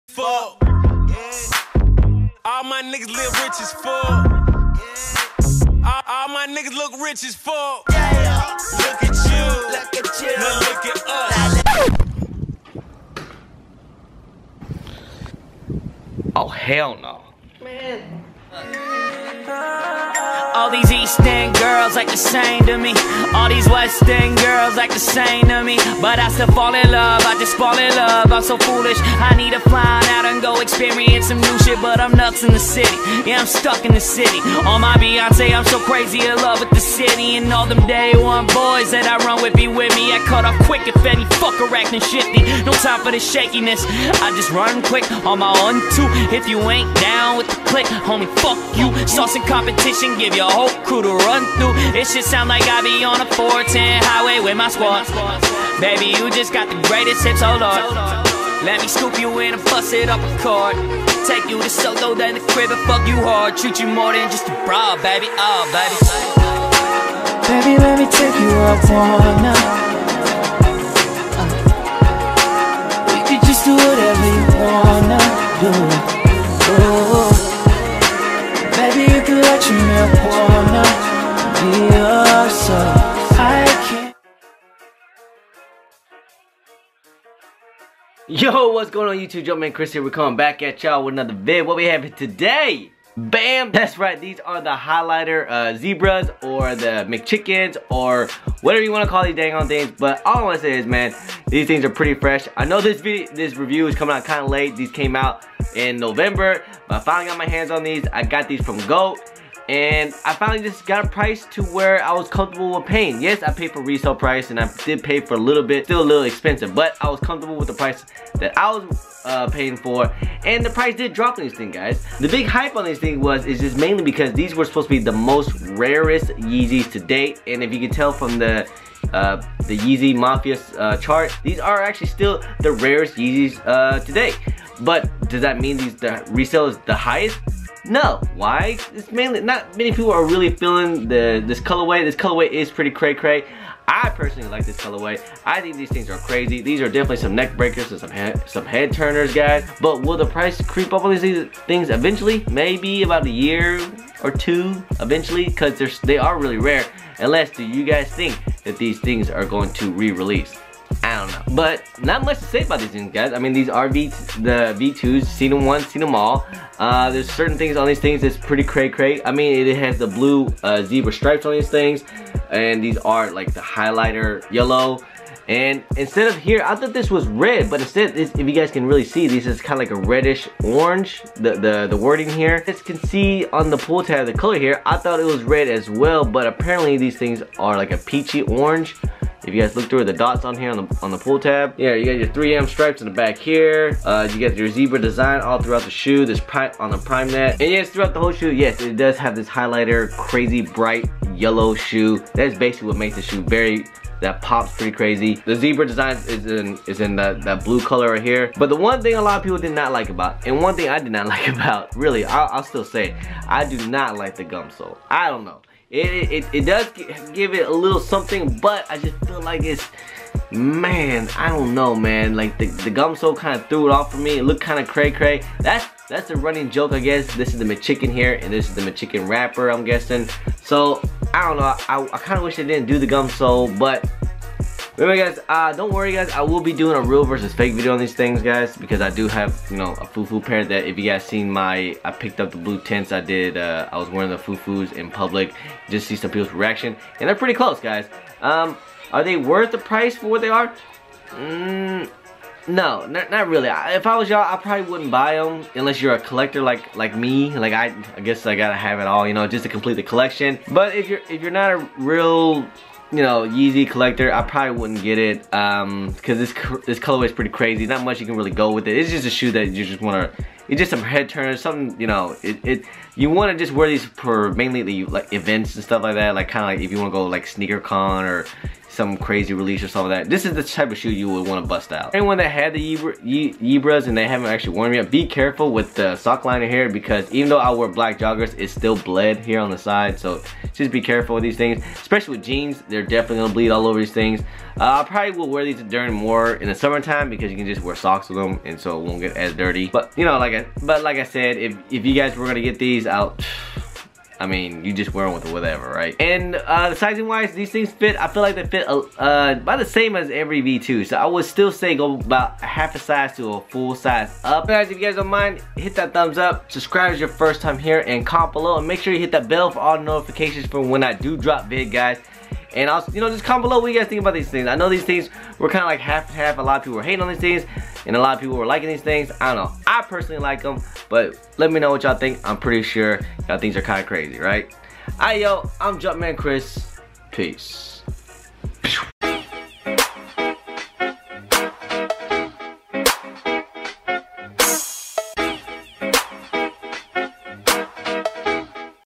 (NOT WORTH $$$) ADIDAS YEEZY V2 "SEMI FROZEN YELLOW" REVIEW & ON FEET
Kik JumpermanAzn

Song